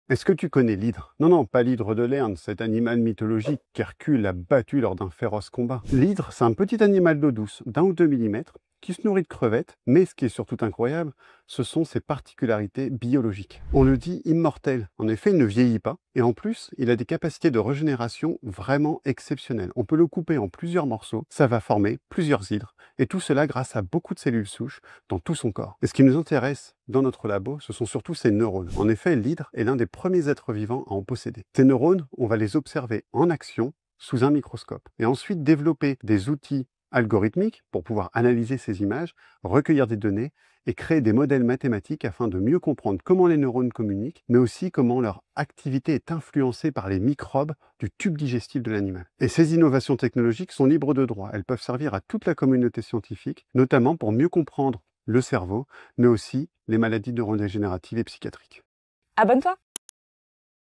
Les secrets de l'hydre
Tu connaissais l’HYDRE ? Pas la mythologique… la vraie. Et elle cache un pouvoir FOU.

Thibault Lagache, chercheur à l’Institut Pasteur, t’embarque à la découverte d’un des organismes les plus fascinants de la planète 🌍👇

👉 Elle ne vieillit pas.👉 Elle peut se régénérer à l’infini.👉 Elle a des neurones… et un microbiote.

Et devine quoi ? Ces mini créatures pourraient bien nous aider à mieux comprendre le CERVEAU humain 🧬

Grâce à l’IA et la modélisation mathématique, Thibault et son équipe analysent comment ces neurones réagissent, communiquent, et interagissent avec les microbes.

🎯 Objectif : explorer les origines de notre système nerveux, et faire avancer la recherche sur les maladies neurodégénératives et psychiatriques.

💡 Et le plus beau ? Tous leurs outils sont en libre accès pour la communauté scientifique.